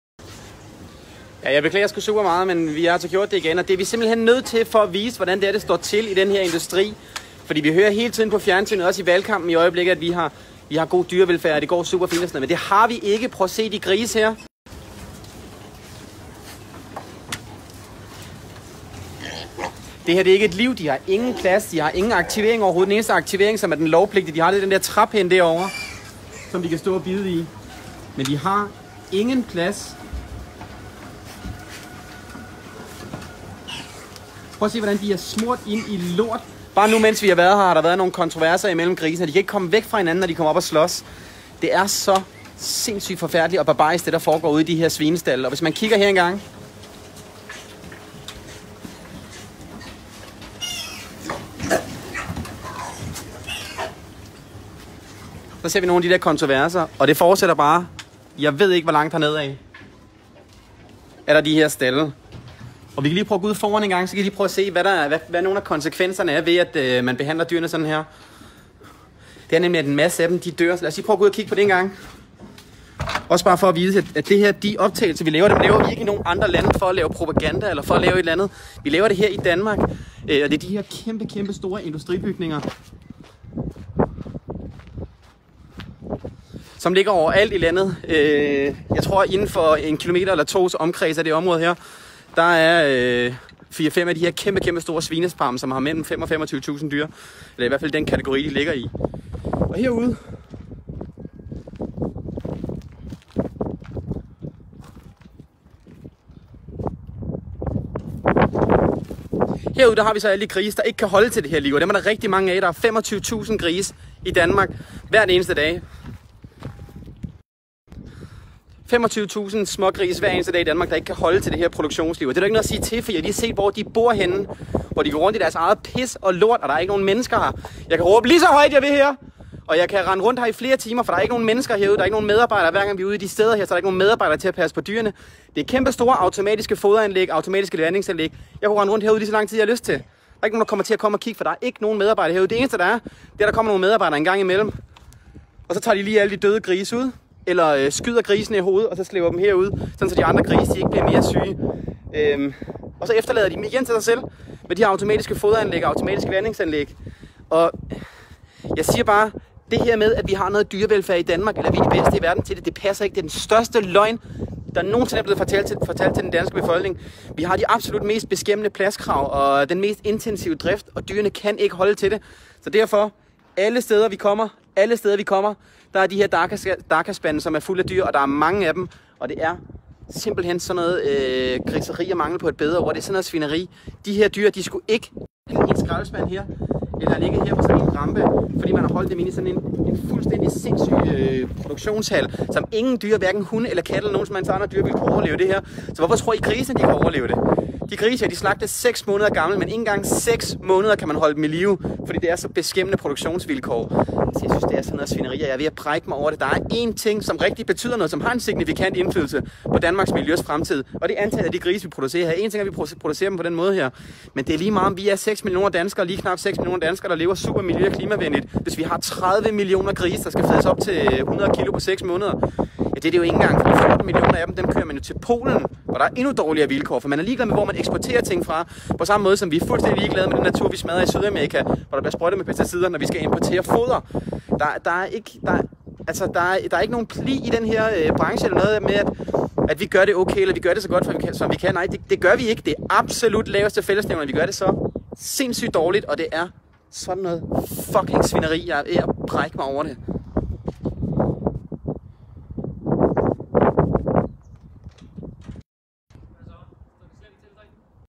0,65 m2 - er det et liv? Hvad sker der?
Vi får at vide at vi har god dyrevelfærd i Danmark. Denne svinefarm viser noget andet, ligesom de mange andre svinefarme vi har været ude og besøge.